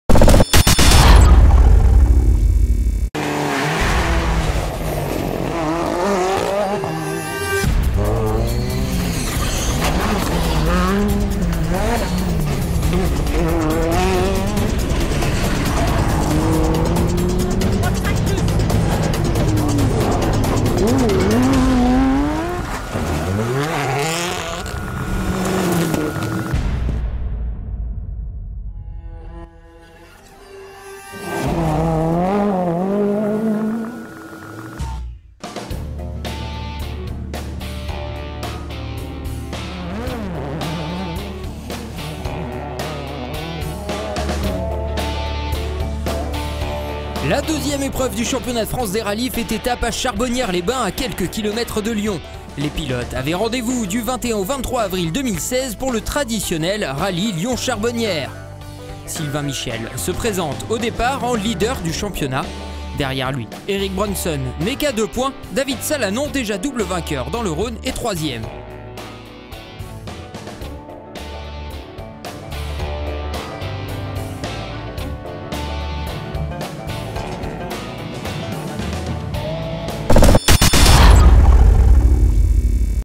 Championnat de France des Rallyes Asphalte 2016 - Lyon-Charbonnières Preview
Vous êtes fans de rallyes ? La deuxième étape du championnat de France des Rallyes Asphalte, sur le célèbre parcours de Lyon-Charbonnières, c'est vendredi 29 avril 2016 à 20h40, sur Motors TV !

Motors TV, La Télévision Grande Vitesse, propose la plus grande offre de programmes dédiés aux sports mécaniques internationaux, avec des évènements exclusifs.